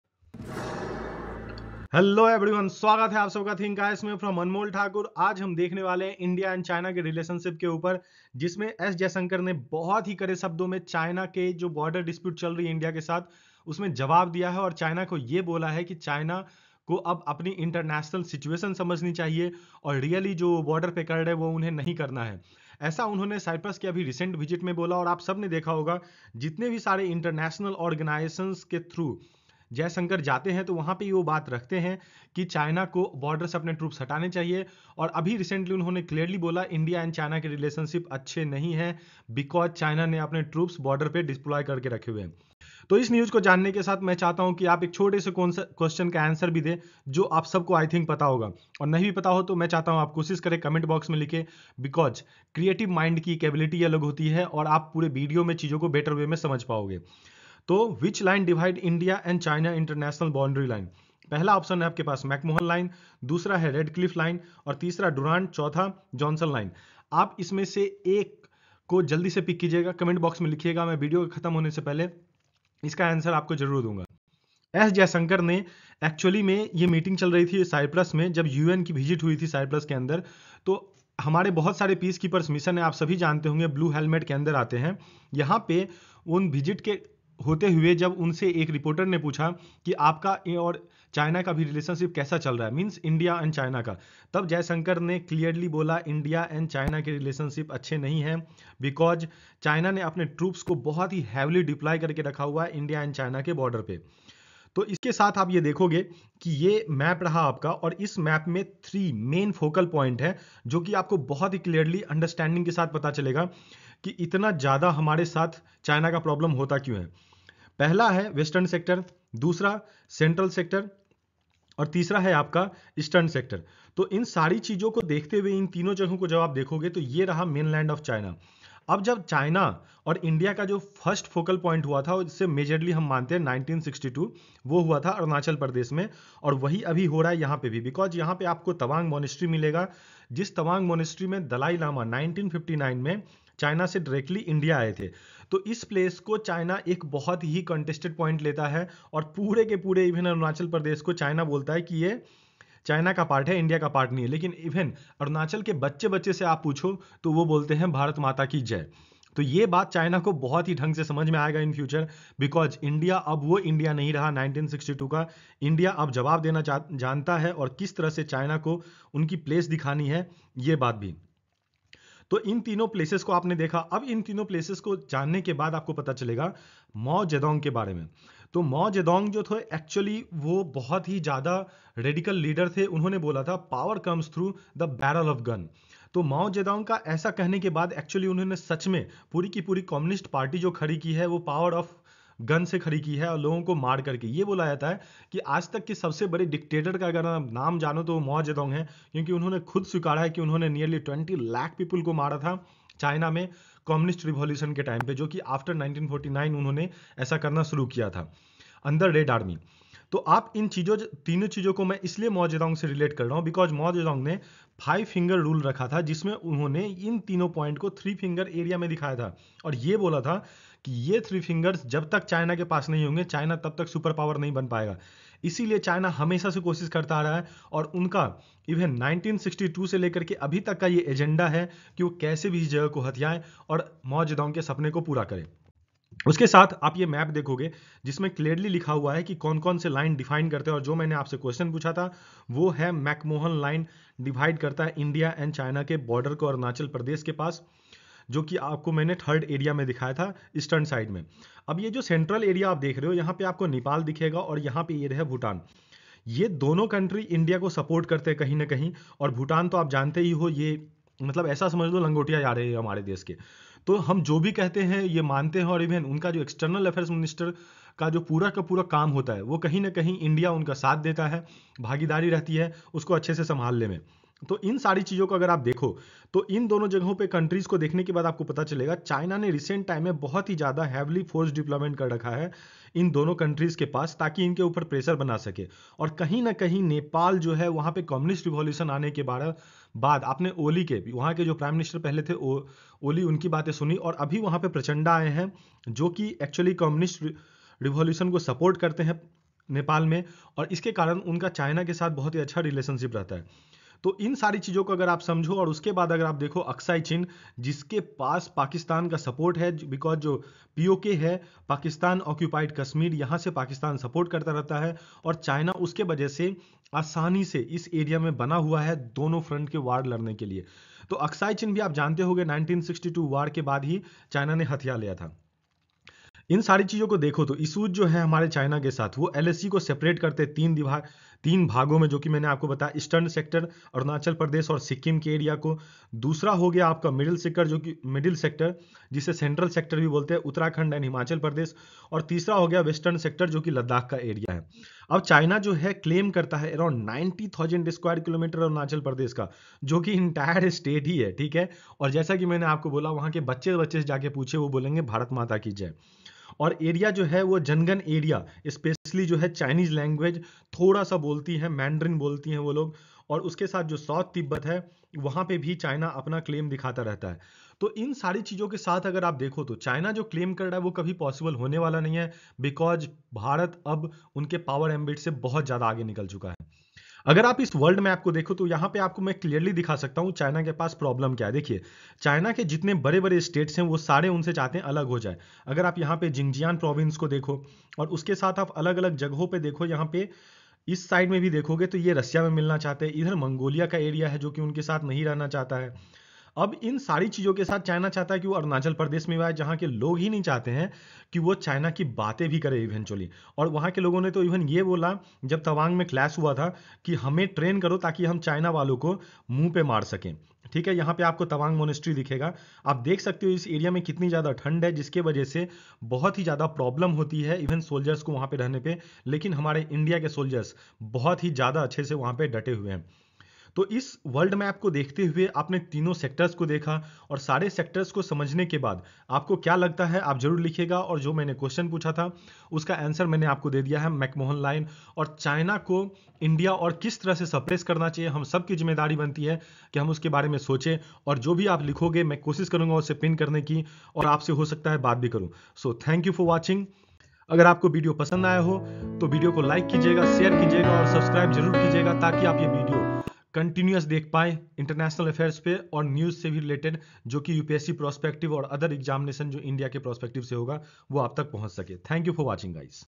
Jaishankar raised India power at Internationally given answer to china |world affairs
videos related to current affairs from India and the world with prospective of UPSC current affairs and other related exam preparation